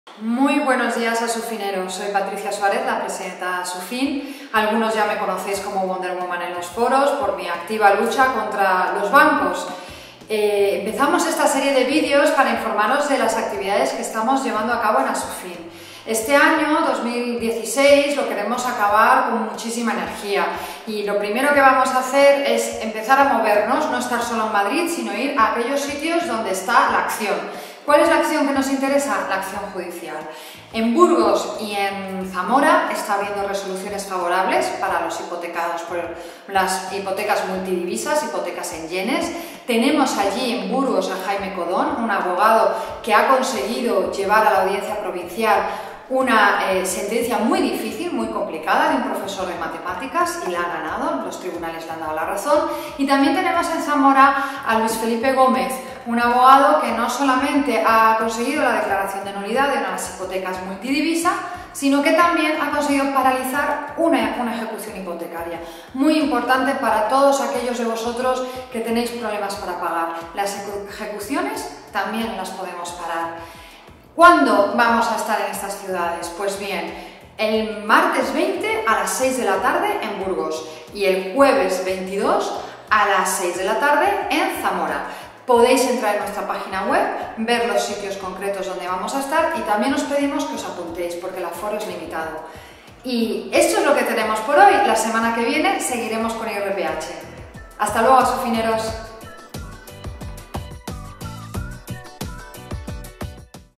ASUFIN VISITA BURGOS Y ZAMORA
¿Afectado por la Hipoteca Multidivisa? Los tribunales de Burgos y Zamora están dando la razón a los afectados por este producto tóxico.

ASUFIN acude a estas ciudades para ofrecer una solución a los afectados. No todo está perdido, apúntate a las conferencias que impartiremos los próximos 20 y 22 de septiembre en Burgos y en Zamora para dar salida a tu problema. Inscripción es gratuita, AFORO LIMITADO.